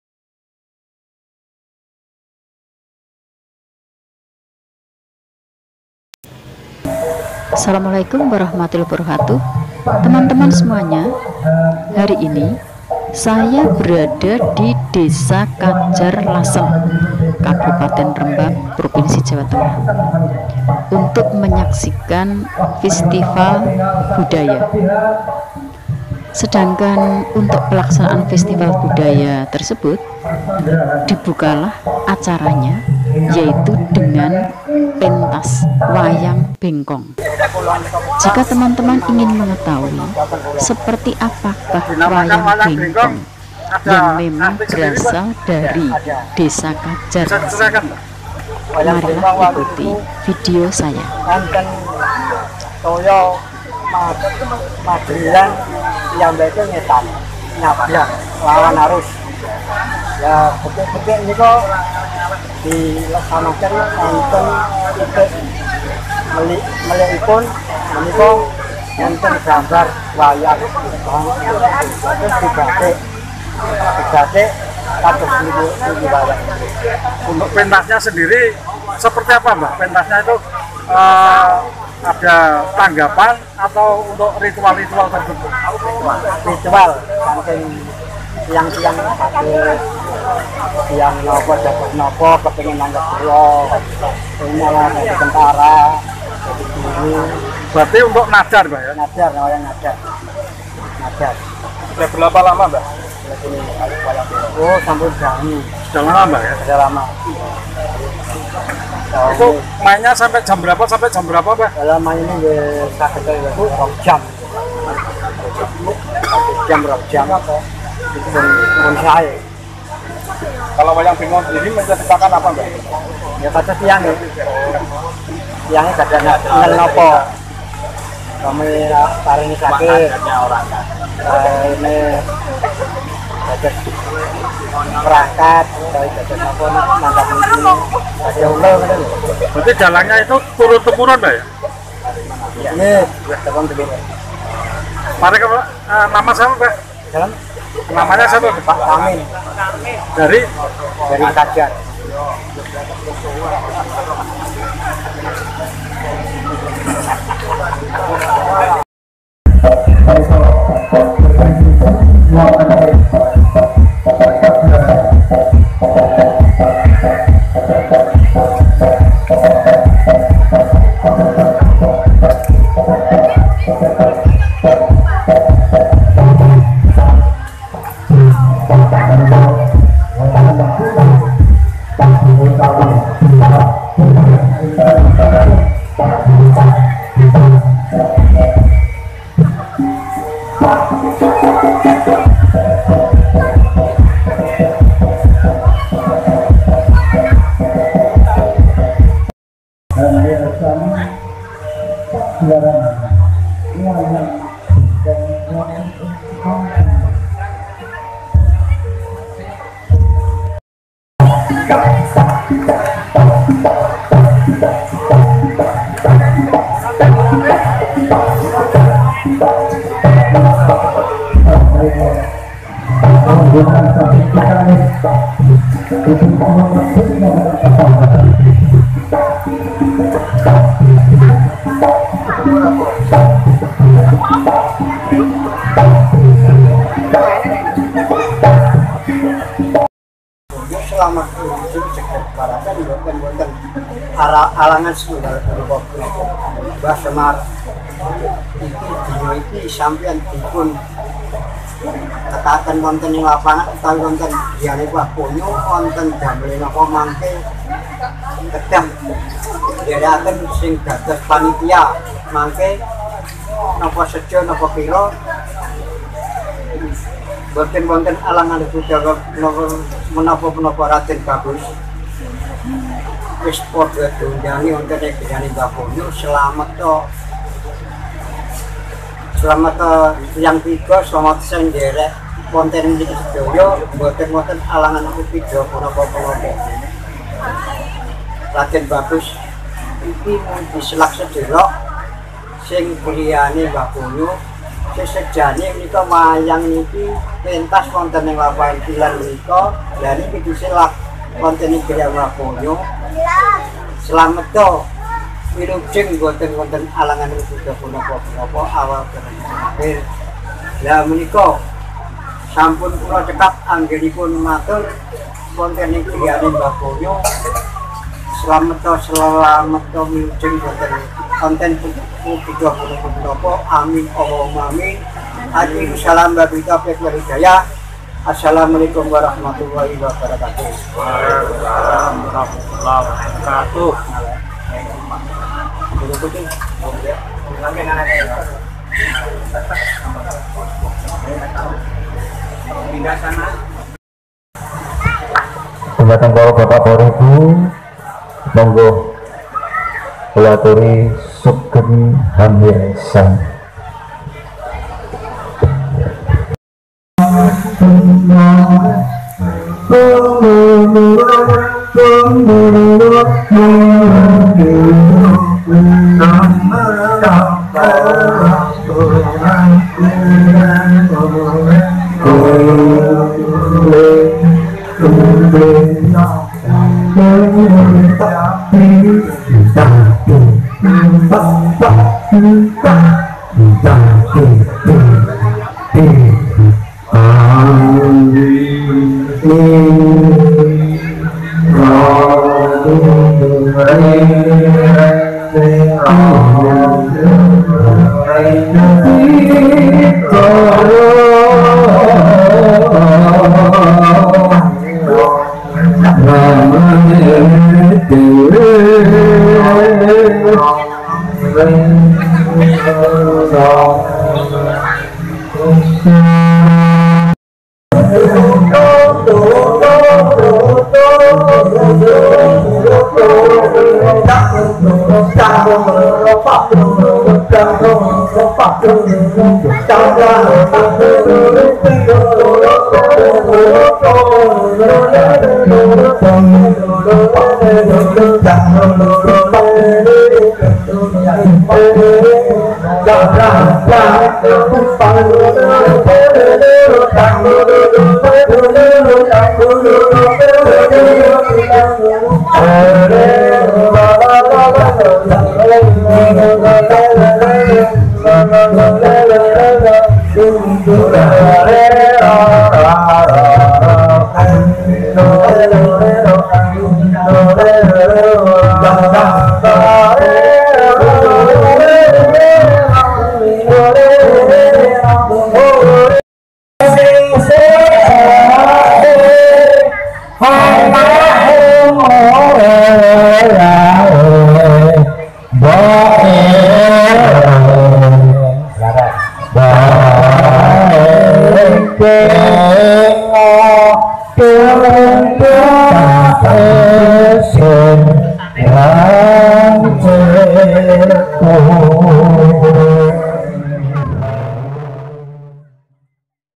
Wayang Bengkong Wayang Mbengkong dari Lasem
Wayang Bengkong merupakan kesenian tradisional asal desa Kajar, Kec. Lasem, Kab. Rembang. Kapan wayang ini mulai mucul dan kenapa dinamai wayang Bengkong kurang begitu jelas ya. Namun wayang ini termasuk wayang yang langka.

Wayang Bengkong juga dinamakan wayang Pekaulan atau wayang Nadzar karena orang yang telah berhasil bernadzar biasanya nanggap wayang ini.

Keunikan wayang Bengkong antara lain ceritanya diambil seputar cerita orang yang menanggapnya dan iringannya memakai mulut yg bersuara seperti gamelan Jawa